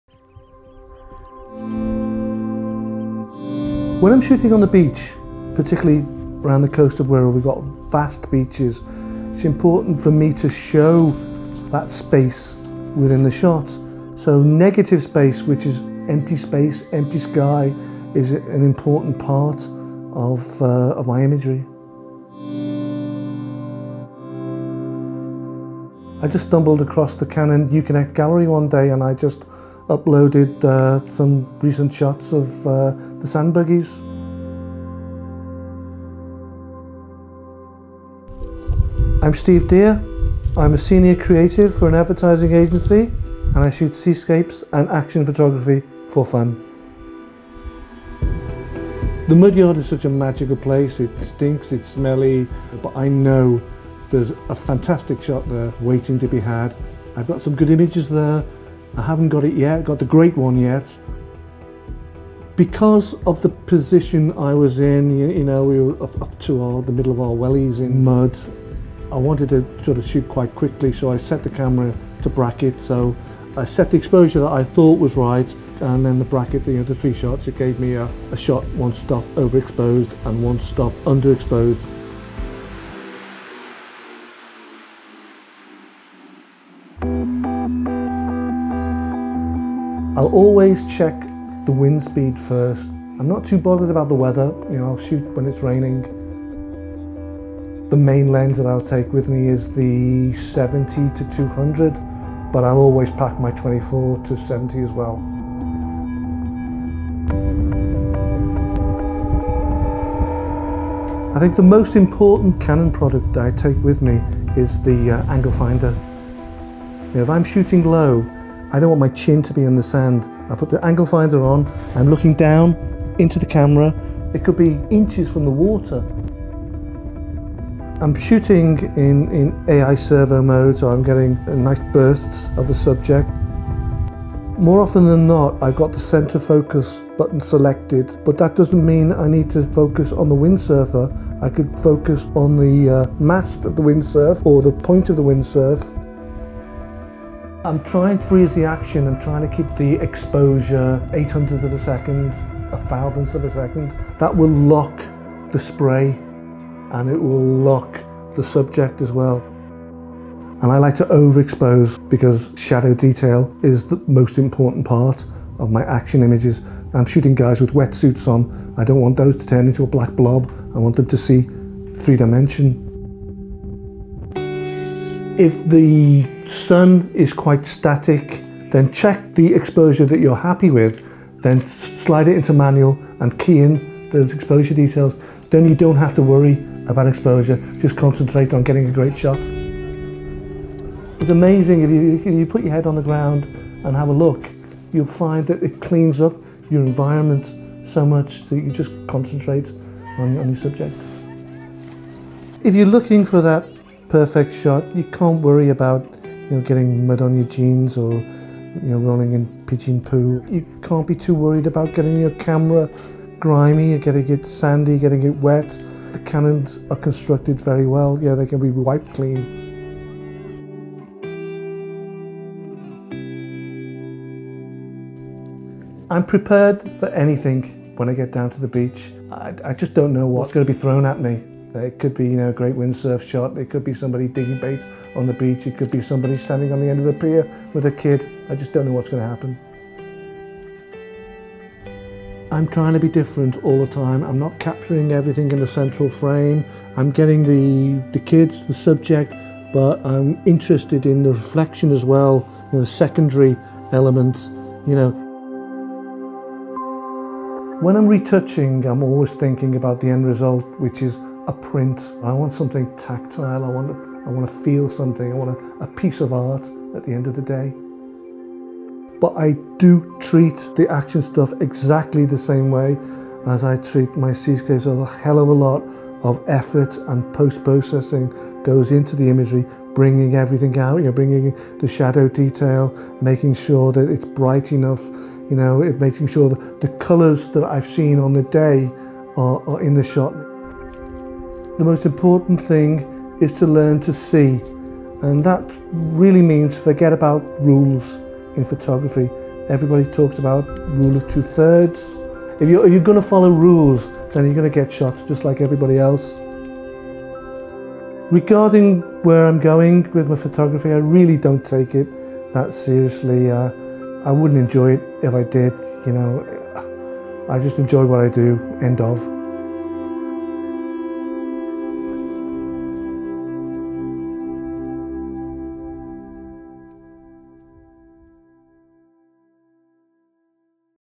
AHK film Steve Deer seascapes and action photography
Senior Advertising Creative, Steve Deer, treats his local neighbourhood on The Wirral in Northwest England as his photographic playground.